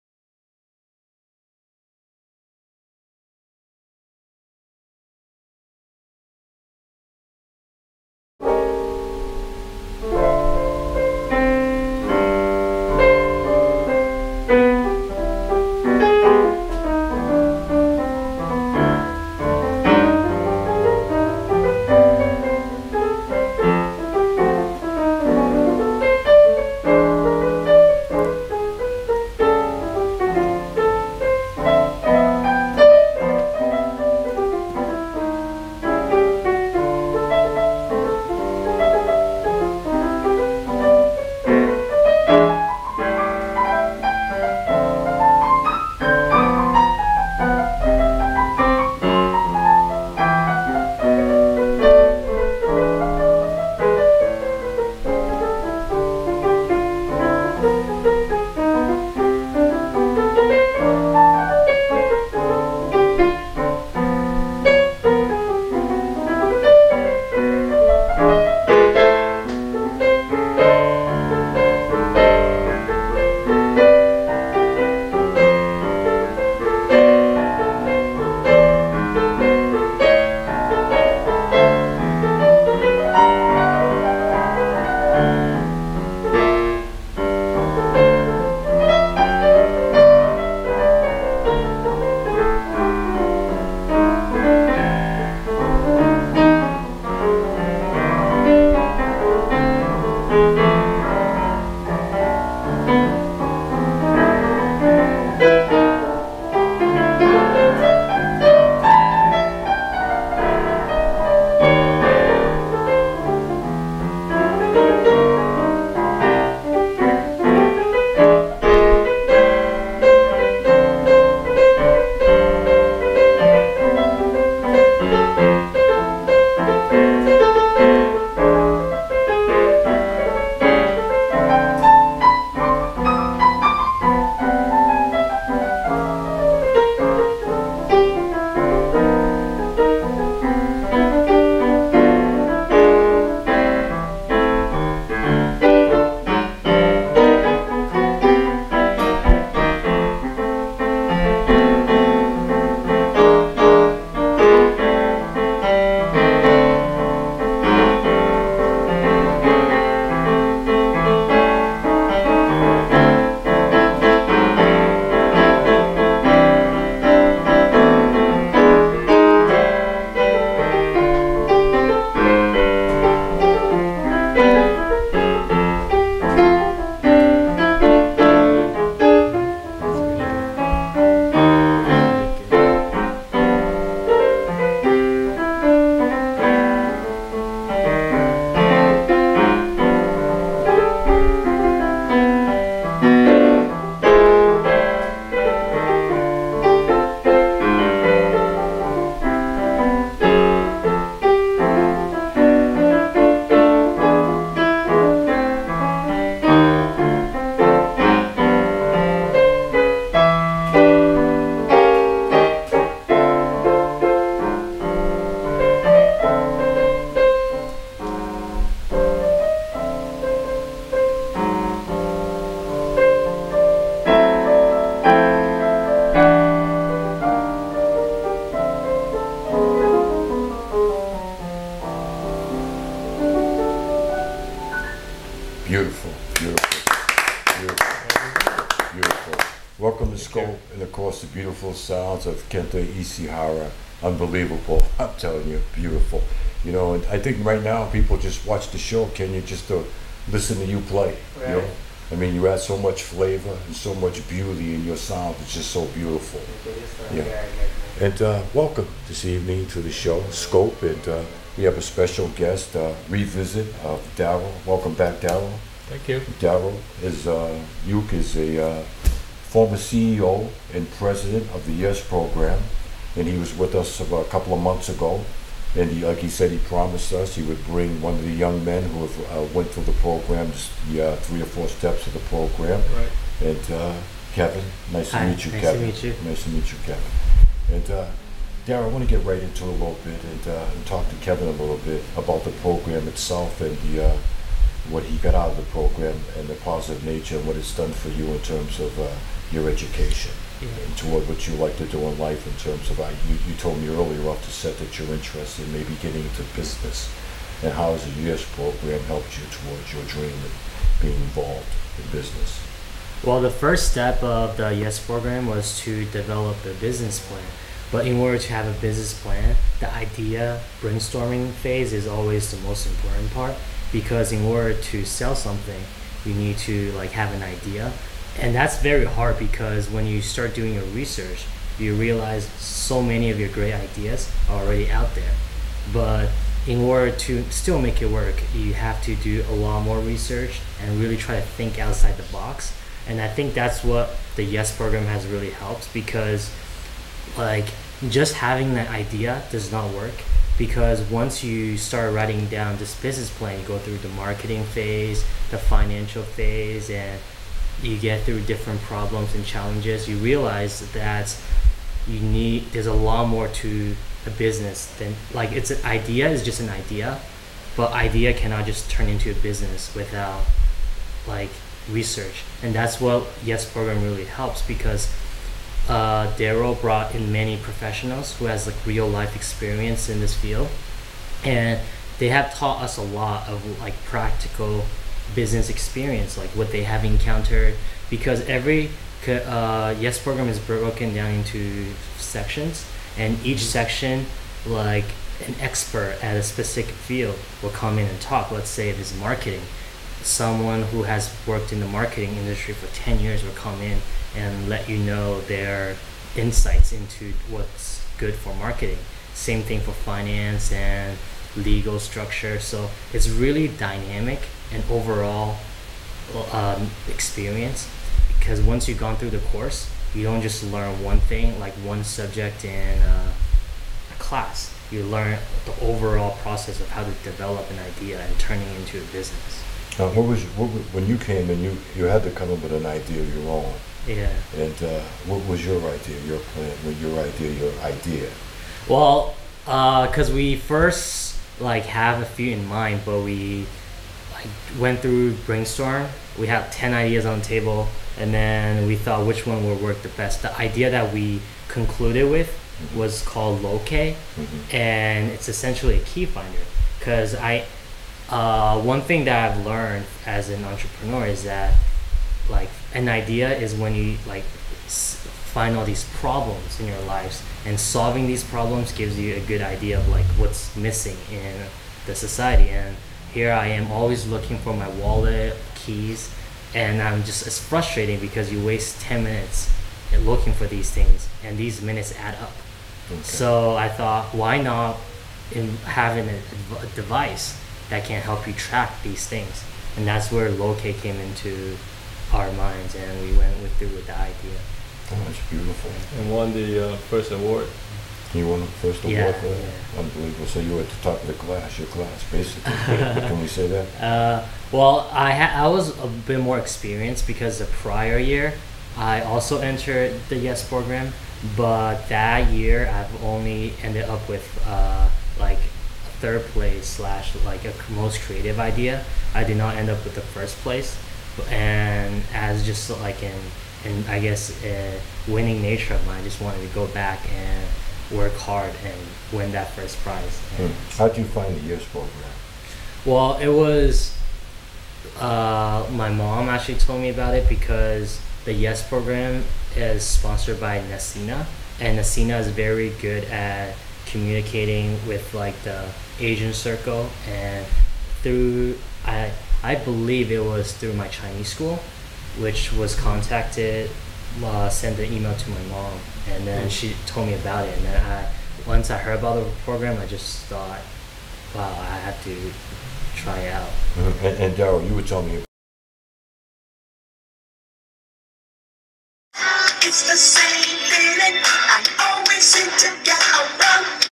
CCTV Cambridge Scope with Clyde, 8/31/2011, YES program from NECINA, Chapter 1
Non-profit Organization, NECINA, provides free and amazing business classes for training high school students and college students to shape and develop entrepreneurial perspectives. Students have chances to feel the real business world before step in . Classes start in the middle of september. it is a very worthy program to spend time on.